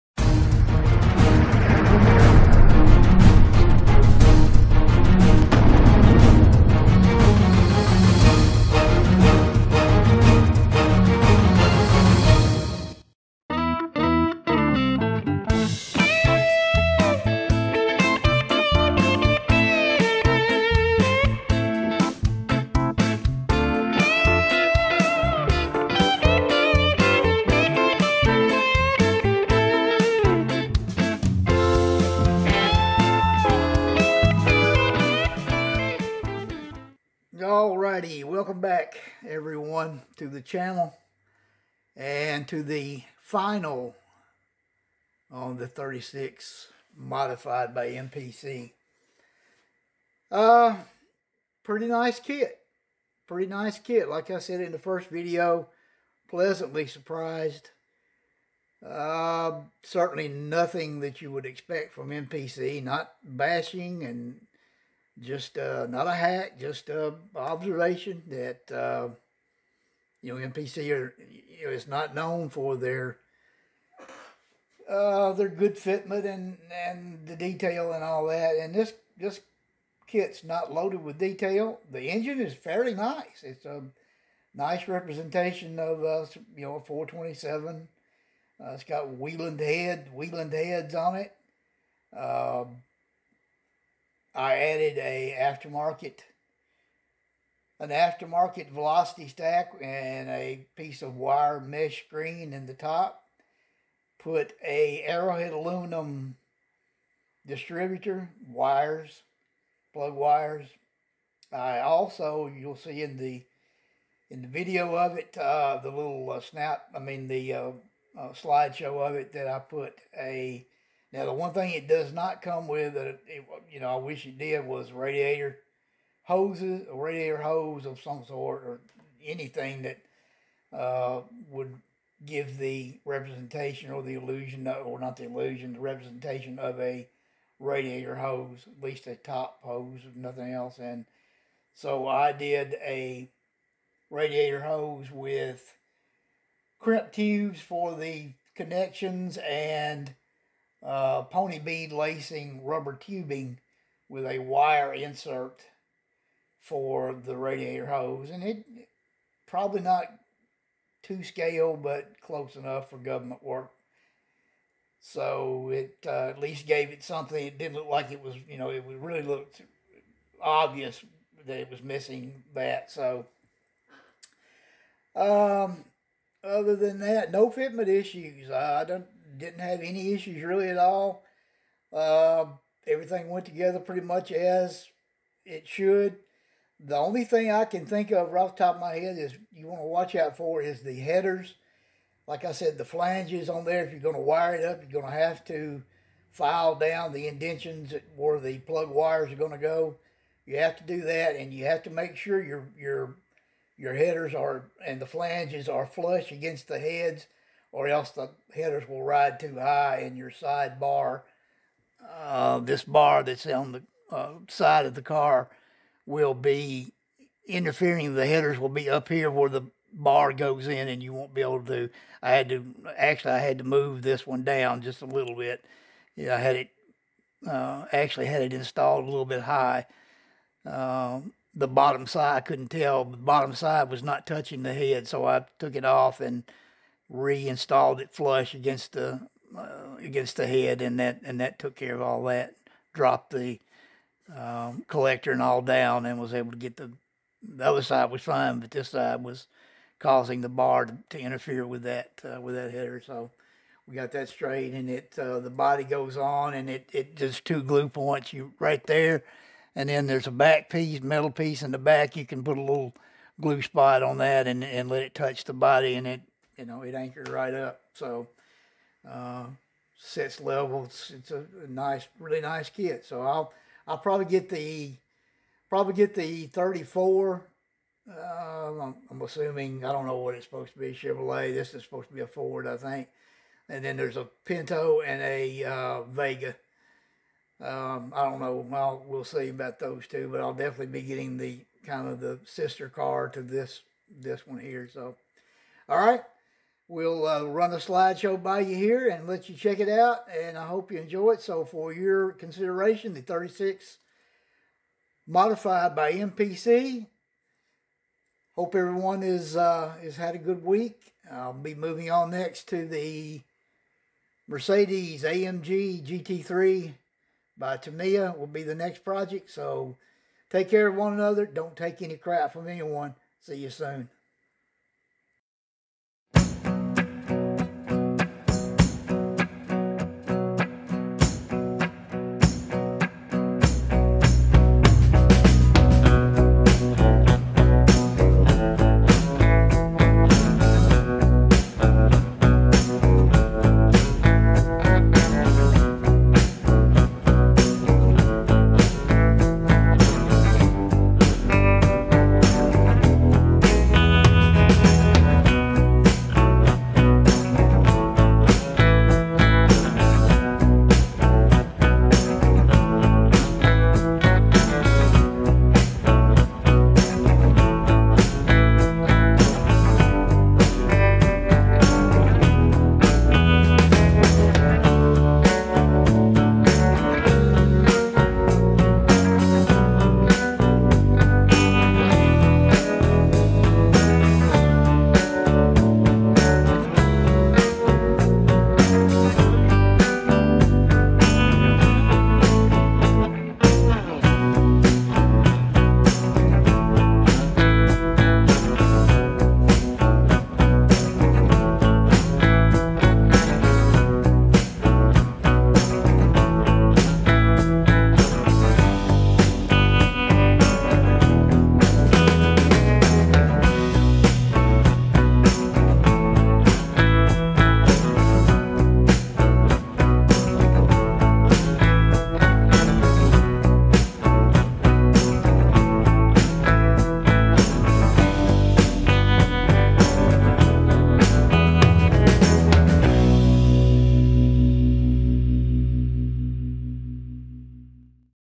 36 Modified Final
This video is the final and reveal for the MPC 36 Modified. This build was a lot of fun. A good slump breaker or something different to break things up.